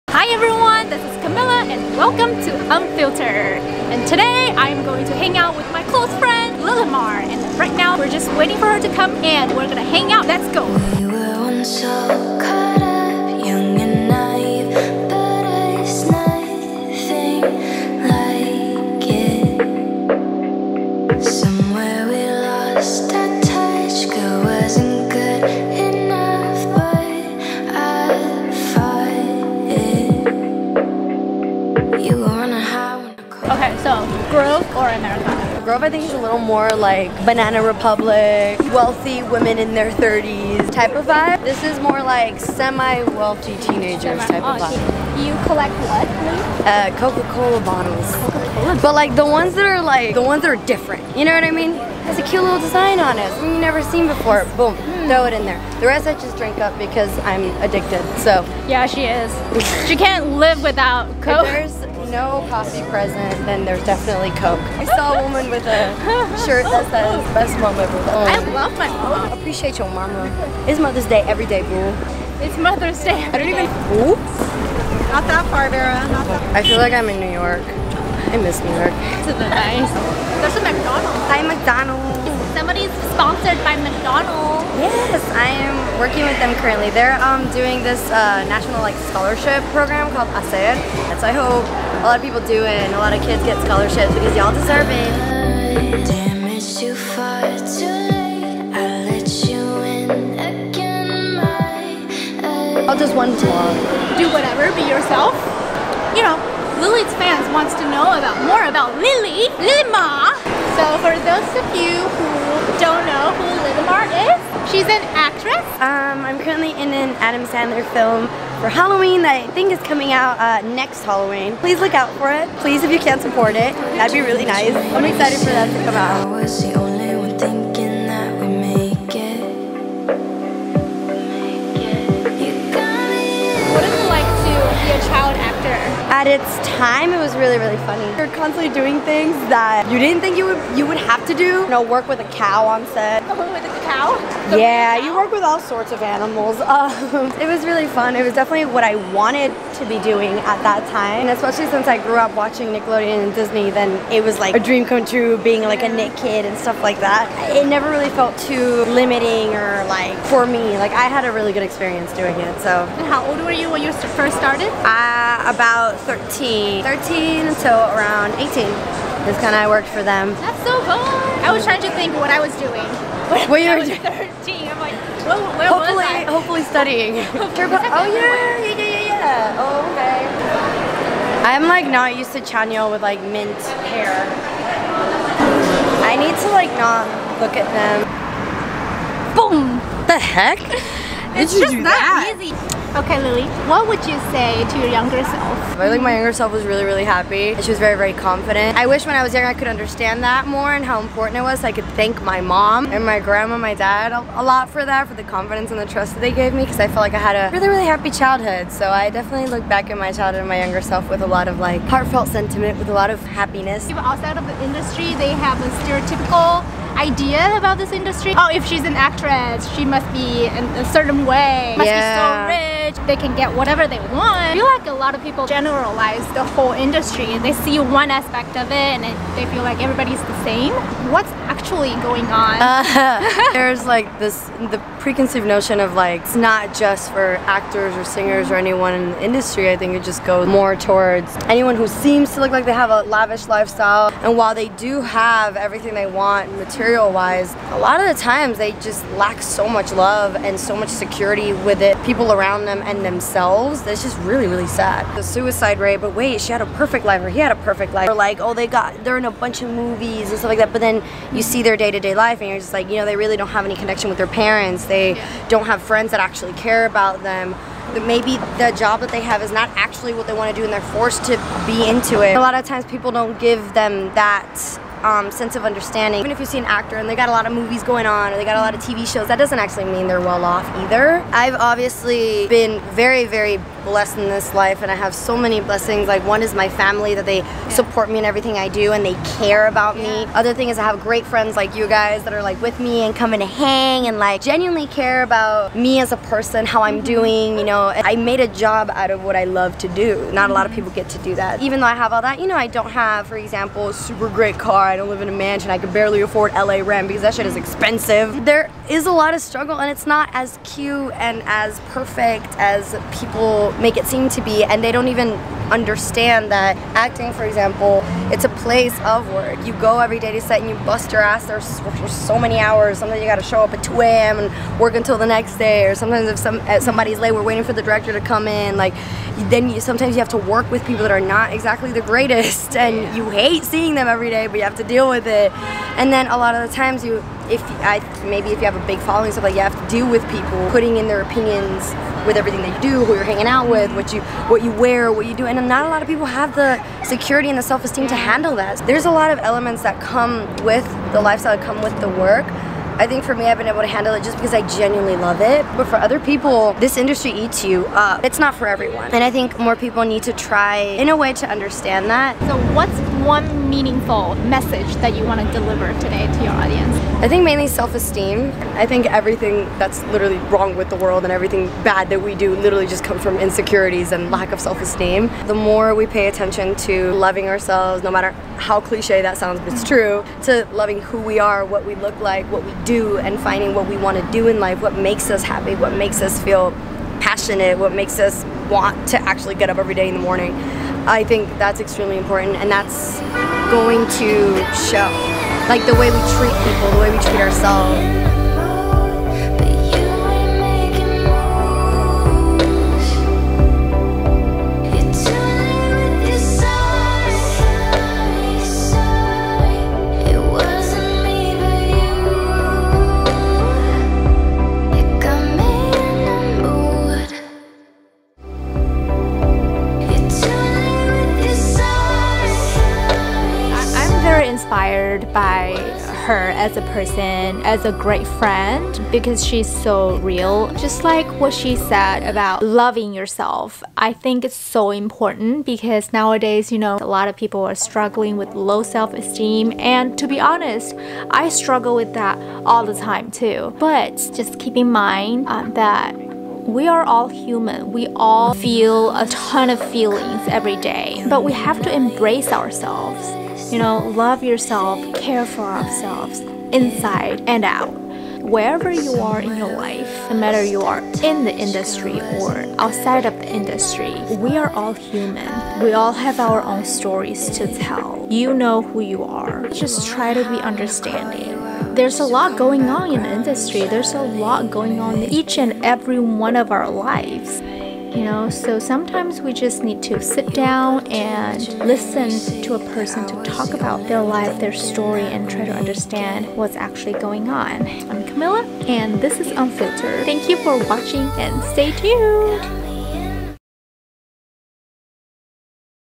Unfilter EP 1: Lilimar opens up about her life as a young Hollywood Actress
Episode 1: Featuring Lilimar

Producer: Camila / @camilwearstyle
Special Guest: Lilimar / @iamlilimar
Executive Producer: Wd / @kidwithapaintbrush

Produced by Shawn Halim / @shawnshalim

Location: Americana
Location: Galleria